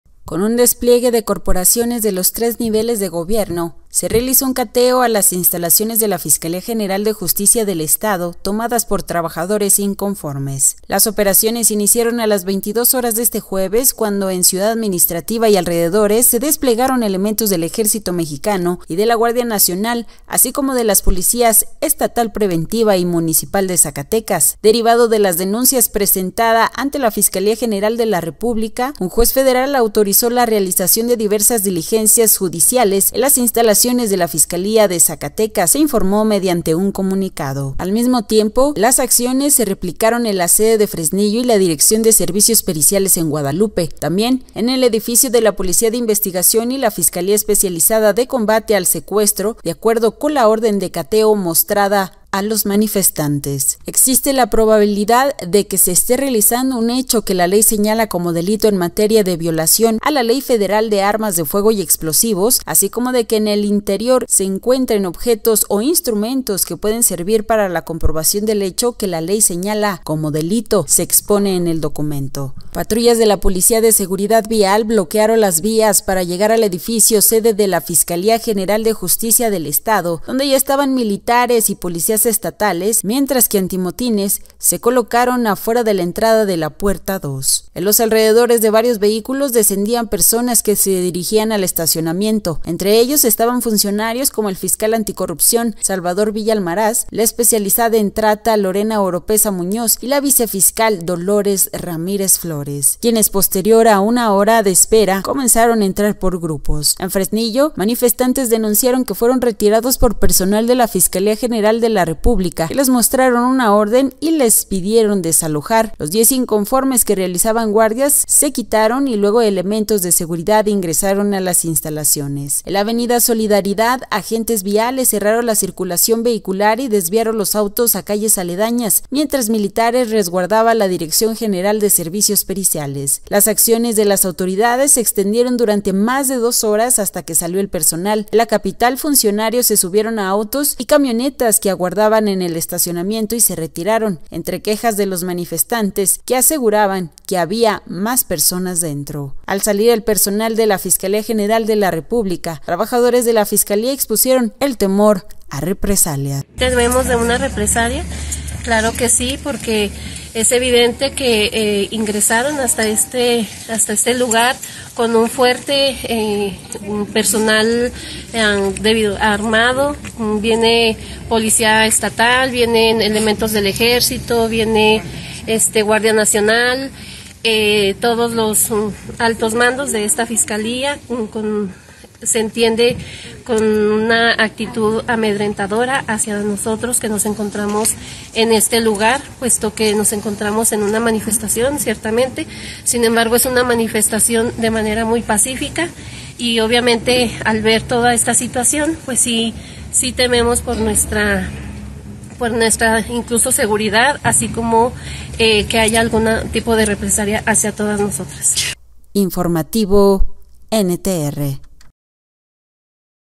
Catean fiscalía
Con un despliegue de corporaciones de los tres niveles de gobierno se realizó un cateo a las instalaciones de la Fiscalía General de Justicia del Estado (FGJE), tomadas por trabajadores inconformes.

Las operaciones iniciaron a las 22 horas de este jueves, cuando en Ciudad Administrativa y alrededores se desplegaron elementos del Ejército Mexicano y de la Guardia Nacional (GN), así como de las policías Estatal Preventiva (PEP) y Municipal de Zacatecas.

“Derivado de la denuncia presentada ante la Fiscalía General de la República (FGR), un juez federal autorizó la realización de diversas diligencias judiciales en las instalaciones de la fiscalía de Zacatecas”, se informó mediante un comunicado.

Al mismo tiempo, las acciones se replicaron en la sede de Fresnillo y la Dirección de Servicios Periciales en Guadalupe. También en el edificio de la Policía de Investigación (PDI) y la Fiscalía Especializada de Combate al Secuestro, de acuerdo con la orden de cateo mostrada a los manifestantes.

“Existe la probabilidad de que se esté realizando un hecho que la ley señala como delito en materia de violación a la Ley Federal de Armas de Fuego y Explosivos, así como de que en el interior se encuentren objetos o instrumentos que pueden servir para la comprobación del hecho que la ley señala como delito”, se expone en el documento.

Llegan funcionarios

Patrullas de la Policía de Seguridad Vial bloquearon las vías para llegar al edificio sede de la FGJE, donde ya estaban militares y policías estatales, mientras que antimotines se colocaron afuera de la entrada de la Puerta 2.

En los alrededores, de varios vehículos descendían personas que se dirigían al estacionamiento. Entre ellas estaban funcionarios como el fiscal anticorrupción, Salvador Villa Almaraz; la especializada en trata, Lorena Oropeza Muñoz, y la vicefiscal, Dolores Ramírez Flores.

“Somos fiscales y vicefiscales. Solo nos avisaron que teníamos que venir”, expusieron en uno de los filtros para acceder al estacionamiento y militares los dejaron pasar, para que se unieran a quienes permanecían afuera de la Puerta 2.

Tras aproximadamente una hora de espera, un elemento de seguridad se asomó desde la entrada al edificio y gritó: “que pase el grupo 1”, por lo que avanzó la mitad y después tocó el turno al “grupo 2”, hasta que todos estuvieron dentro.

“¿Todos ellos estaban autorizados para el cateo?”, “son como 50 personas”, cuestionaban frente a los antimotines trabajadores que se mantienen en paro. Otros más grababan con sus celulares lo que podía verse a través de las paredes de cristal.

Lo mismo hacía el personal de la Comisión de Derechos Humanos del Estado de Zacatecas (CDHEZ), que acudió a observar las acciones.

Adentro, los funcionarios se acomodaron en círculo y así permanecieron un momento, en tanto hablaban, luego subieron por las escaleras y volvieron a bajar, permaneciendo en la planta baja.

Sin información

En Fresnillo, manifestantes denunciaron que fueron retirados por personal de la Fiscalía General de la República (FGR), que les mostraron una orden y les pidieron desalojar. Los 10 inconformes que realizaban guardias se quitaron y luego elementos de seguridad ingresaron a las instalaciones.

“No sabemos qué pasará, no se nos ha hecho del conocimiento nada. Para que nos informen hay que esperar a que digan qué es lo que se hizo allá adentro”, expuso uno de los manifestantes, quien también denunció que aún no recibían en pago de su quincena.

En la avenida Solidaridad, agentes viales cerraron la circulación vehicular y desviaron los autos a calles aledañas, mientras militares resguardaban la Dirección General de Servicios Periciales.

Las acciones de las autoridades se extendieron durante más de dos horas hasta que salió el personal. En la capital, funcionarios se subieron a autos y camionetas que aguardaban en el estacionamiento y se retiraron, entre quejas de los manifestantes, que aseguraban que había más personas dentro.

“Tememos por nuestra seguridad y que haya algún tipo de represalia, pero seguimos en nuestro movimiento. Estamos en una lucha que vamos a continuar porque creemos que es un derecho exigir un mejor sueldo, una mejor calidad laboral, porque estamos cansados y vamos a continuar alzando la voz hasta que seamos escuchados”, afirmaron manifestantes.

La FGJE informó que los avances de la investigación que puedan ser públicos serán informados a la ciudadanía por los canales oficiales institucionales, por parte de las autoridades competentes.